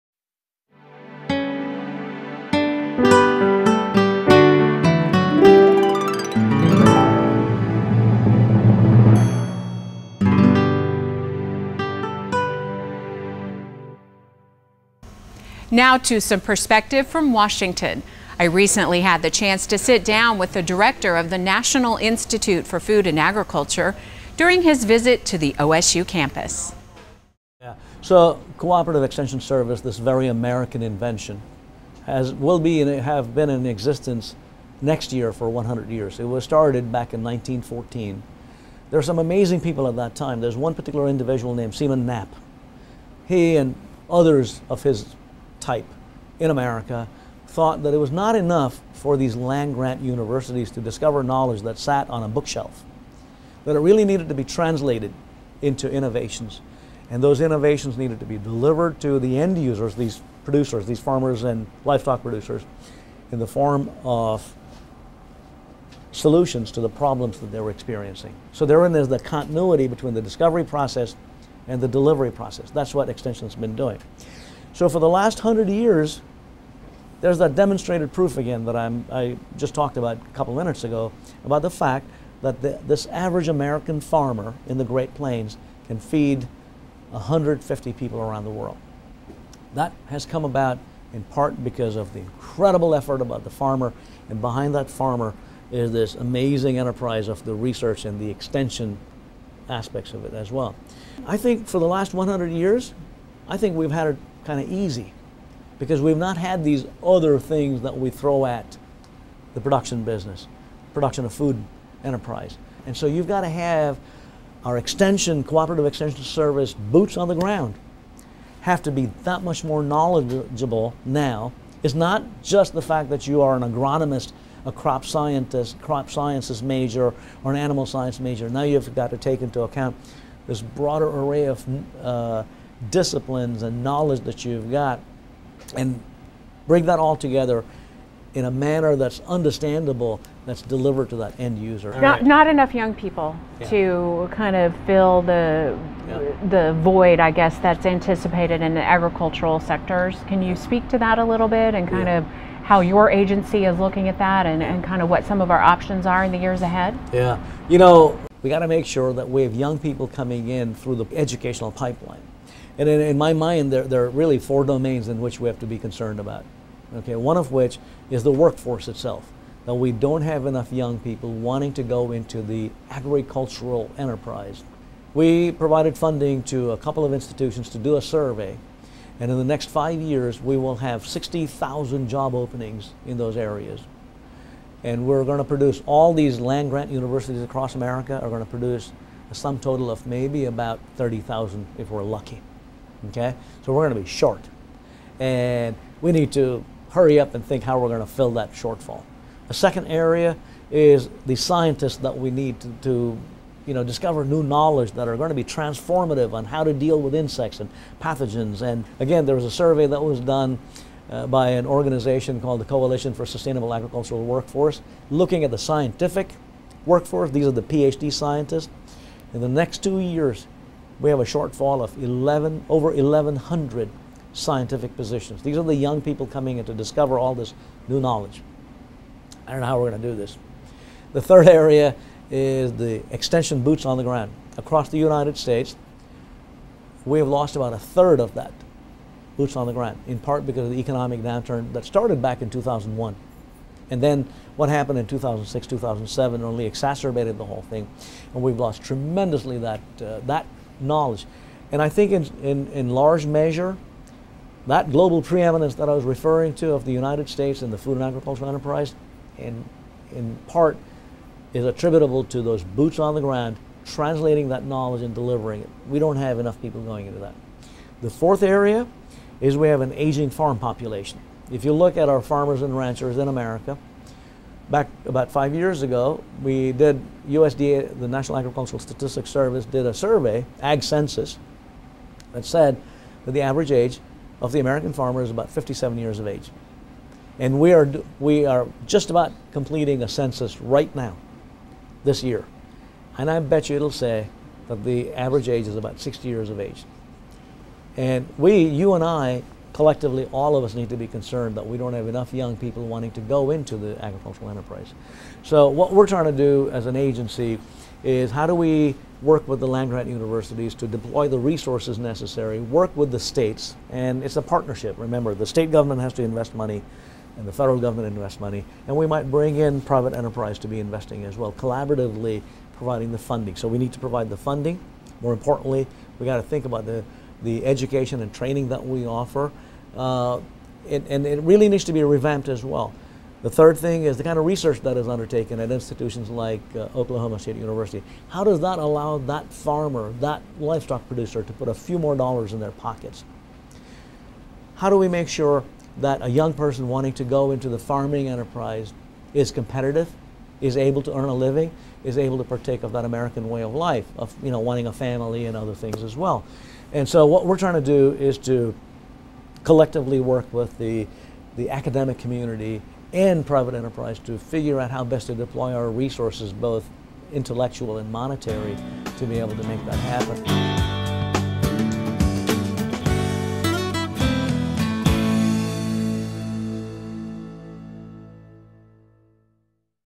NIFA Director (9/28/13)
We have a one-on-one interview with Sonny Ramaswamy, director of the USDA's National Institute of Food and Agriculture, during his recent visit to OSU.